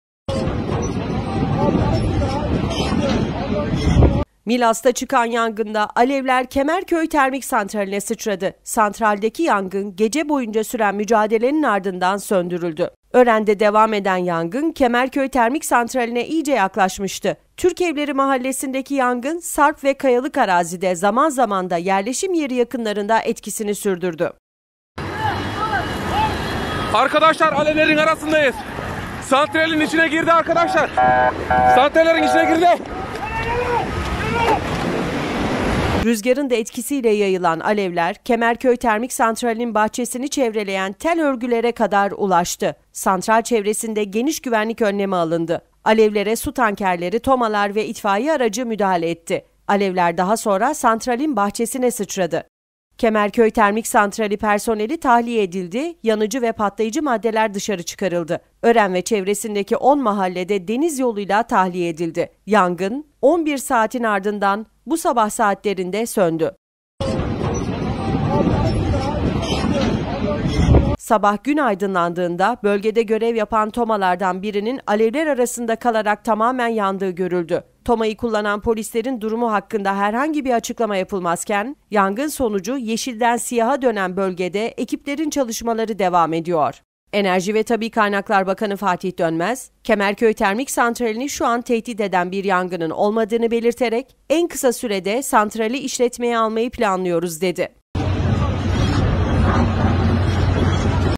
YANGIN TERMİK SANTRALE SIÇRADI
Yangın Termik Santrale Sıçradı

Milas’ta çıkan yangında alevler Kemerköy Termik Santraline sıçradı. Santraldeki yangın, gece boyunca süren mücadelenin ardından söndürüldü.

Ören'de devam eden yangın Kemerköy Termik Santrali'ne iyice yaklaşmıştı. Türkevleri Mahallesi'ndeki yangın sarp ve kayalık arazide, zaman zaman da yerleşim yeri yakınlarında etkisini sürdürdü.

Rüzgarın da etkisiyle yayılan alevler, Kemerköy Termik Santrali'nin bahçesini çevreleyen tel örgülere kadar ulaştı. Santral çevresinde geniş güvenlik önlemi alındı. Alevlere su tankerleri, TOMA'lar ve itfaiye aracı müdahale etti. Alevler, daha sonra santralin bahçesine sıçradı.

Kemerköy Termik Santrali personeli tahliye edildi, yanıcı ve patlayıcı maddeler dışarı çıkarıldı. Ören ve çevresindeki 10 mahalle de deniz yoluyla tahliye edildi. Yangın, 11 saatin ardından bu sabah saatlerinde söndü.

Sabah gün aydınlandığında bölgede görev yapan TOMA'lardan birinin alevler arasında kalarak tamamen yandığı görüldü. TOMA'yı kullanan polislerin durumu hakkında herhangi bir açıklama yapılmazken, yangın sonucu yeşilden siyaha dönen bölgede ekiplerin çalışmaları devam ediyor.

Enerji ve Tabii Kaynaklar Bakanı Fatih Dönmez, Kemerköy Termik Santrali'ni şu an tehdit eden bir yangının olmadığını belirterek, "En kısa sürede santrali işletmeye almayı planlıyoruz" dedi.